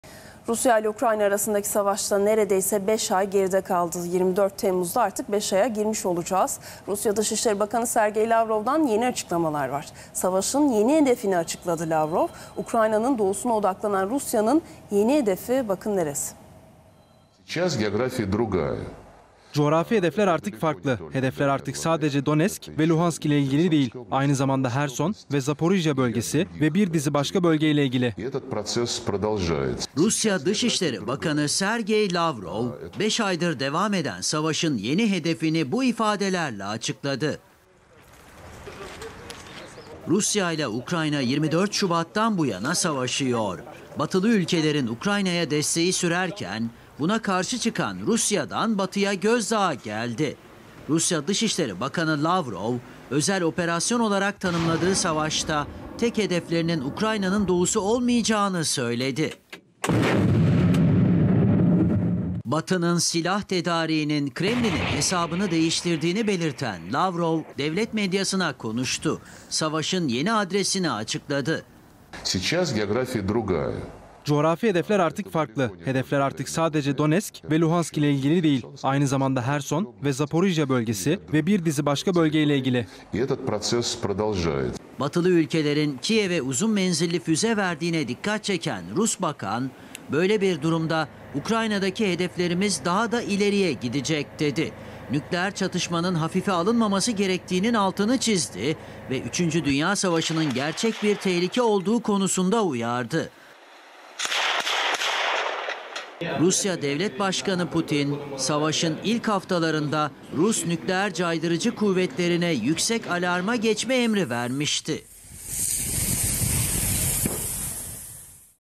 Rusya ülkenin güneyini de hedefe koydu
Ukrayna'daki savaş devam ederken Rusya Dışişleri Bakanı Sergey Lavrov'dan tabiri caizse gündeme bomba gibi düşen sözler geldi. Lavrov, Rusya'nın Ukrayna'daki yeni hedefini açıkladı.